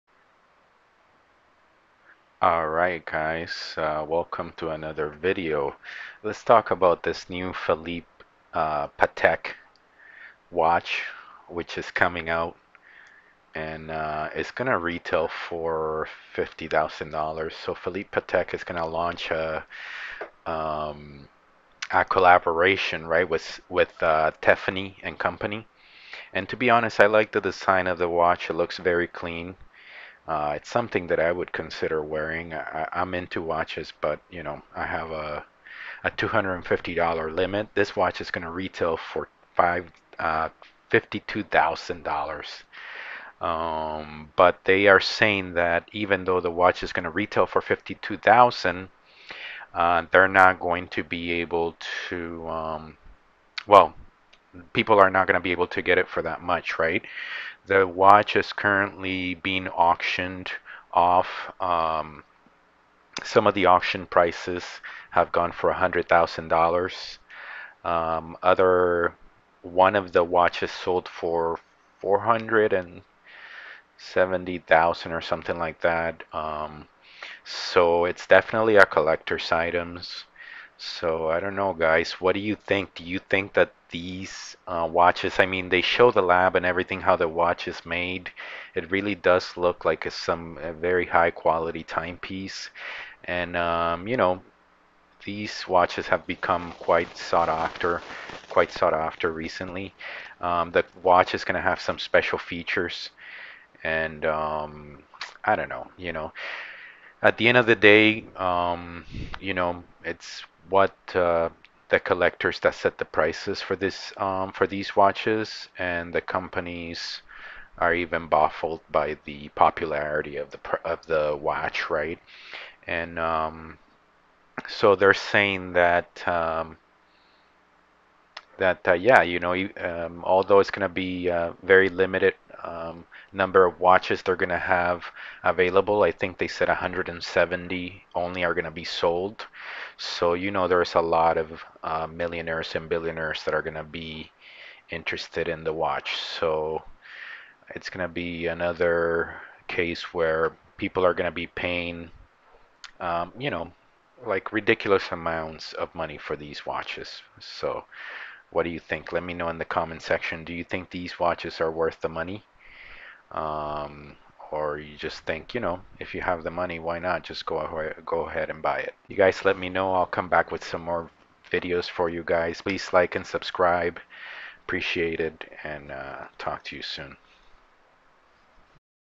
New $50K Patek Philippe Tiffany & Co Special Edition Watch: Auctioned for $470K (REACTION VIDEO)
New $50K Patek Philippe and Tiffany & Co Special Edition Nautilus Ref. 5711 Watch:  Auctioned for $470K  (REACTION  VIDEO)

"Many watch buyers were enraged. Getting a Ref. 5711, which retailed for around $30,000, already involved long waiting lists and opaque terms from dealers, even for preferred Patek customers. Now, buying a new 5711 would be impossible. Prices for pre-owned Ref. 5711s — already been prone to speculation — soared to more than $150,000 online. One sold at auction in 2020 at Sotheby’s for $475,000.

Patek Philippe announced Monday that it is making 170 special versions of its most popular watch as part of a partnership with Tiffany & Co., a launch likely to set off a buying frenzy among wealthy watch collectors.

The surprise move comes less than a year after Patek discontinued the famed watch, known as the Nautilus Ref. 5711, causing demand and prices to skyrocket.

Patek said it is making 170 special Ref. 5711 watches with a Tiffany-blue face that will be sold at select Tiffany boutiques. The watch was created to honor the 170-year partnership between the Swiss watchmaker and Tiffany, as well as Tiffany’s new ownership under French luxury giant LVMH, Patek Philippe said." -- cnbc